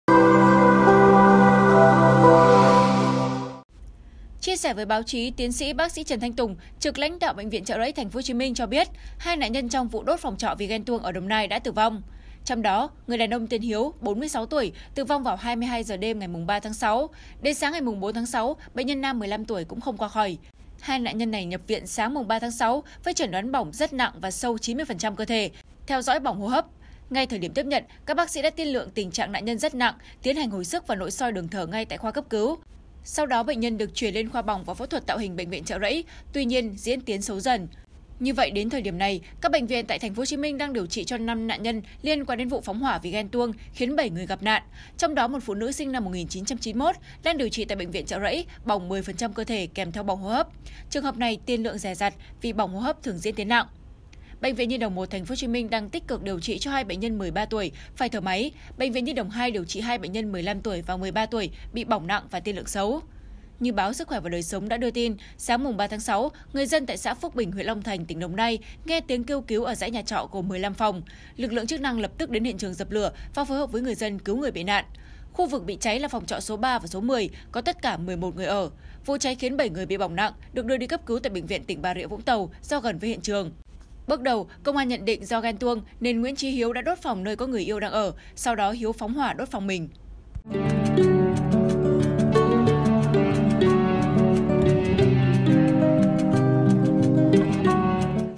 Vụ Đốt Nhà Trọ Vì Ghen Ở Đồng Nai: Nghi Phạm Tử Vong I SKĐS
SKĐS | Các nạn nhân vào Bệnh viện Chợ Rẫy trong tình trạng bỏng rất sâu và nặng, tiên lượng khó qua khỏi ngay từ đầu. Trong số 2 ca tử vong có một người tên Hiếu 46 tuổi, là nghi phạm đã phóng hỏa.